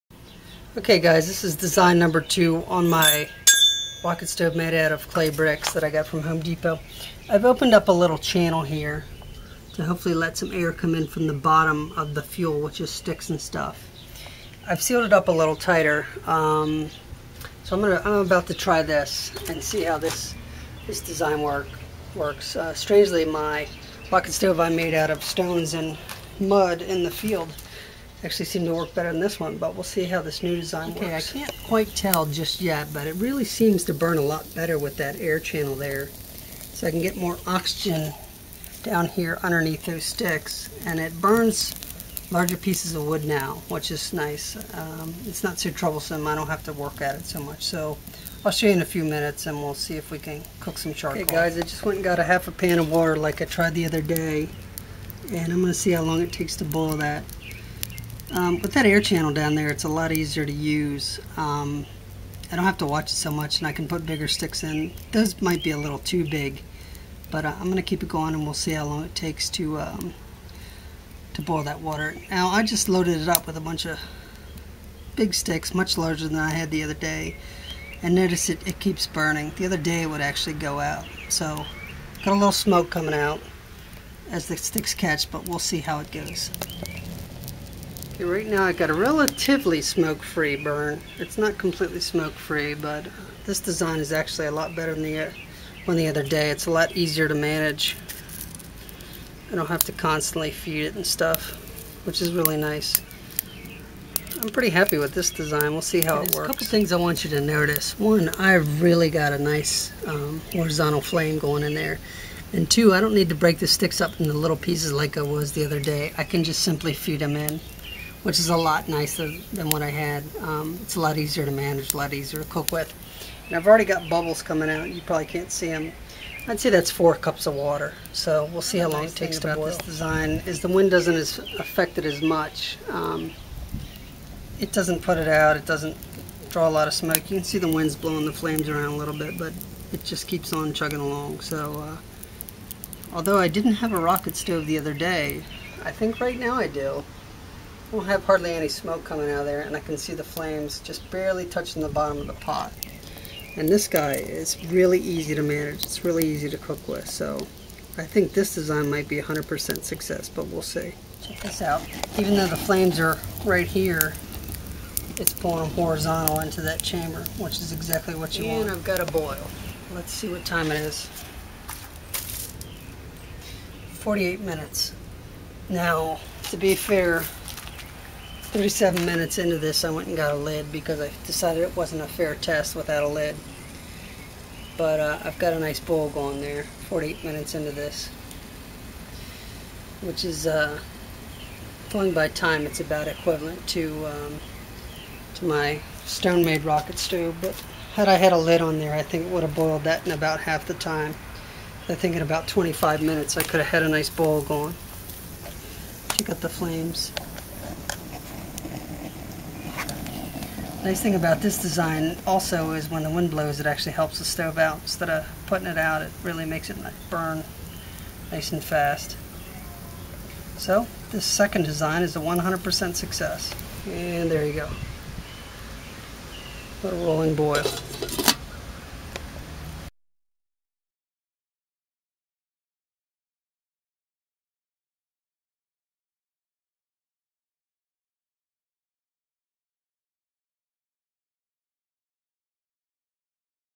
Second Design of 20 Clay brick Rocket stove-100% success
I added an air channel to this version that allowed oxygen to come into the fuel(sticks) from underneath the flame, which made this stove a lot more stable, and easy to use.  The wind didn't effect this design like the first version.  On the first attempt, when the wind blew it would smolder out the fire, by destroying the natural draft.  I would get smoke coming out the front, and the fire would go out.  This design is good, but as always and with everything, it could use some improvement.  With better insulation in the heat riser, I might be able to burn even larger stock.  I don't know if more air, would actually also help me burn bigger sticks.  You must occasionally clean out that channel underneath your fire, as sticks and coals fall into it, but it works pretty well, and much better than attempt one...